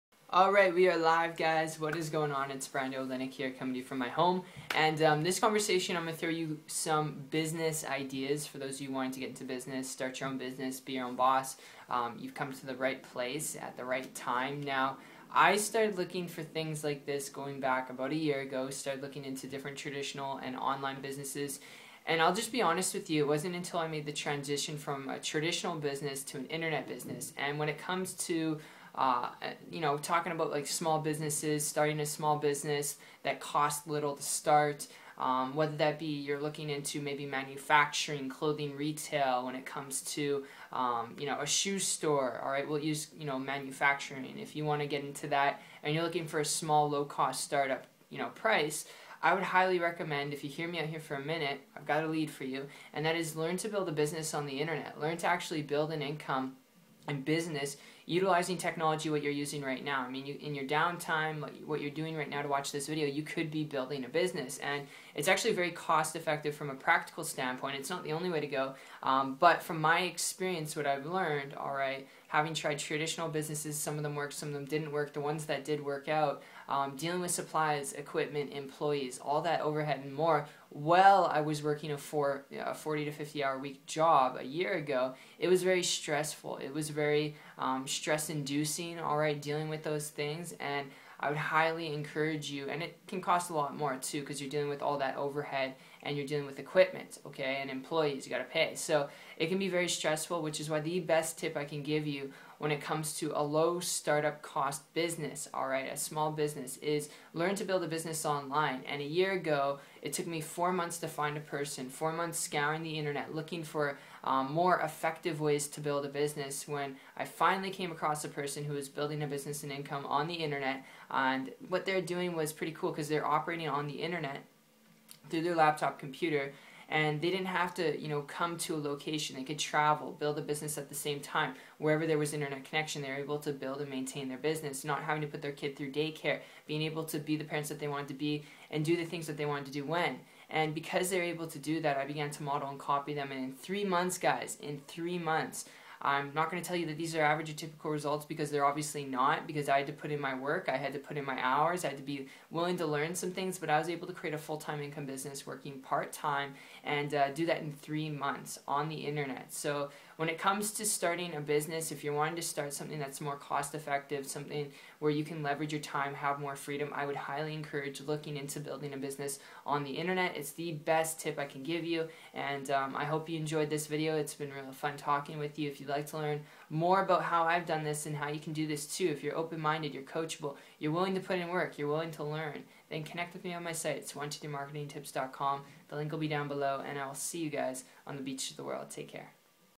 Small Manufacturing Business Ideas That Cost Little To Start - Review
- Small Manufacturing Business Ideas That Cost Little To Start - Here's a couple different tips on starting a low cost business and much more.

In this video review I'll be covering not just the aspect of "small manufacturing business ideas that cost little to start" but really diving into my personal experience on what I've learned form people who have made lot's of money building a business online.

It's been super fun talking about small manufacturing business ideas that cost little to start I hope you enjoyed this if you made it to the very end.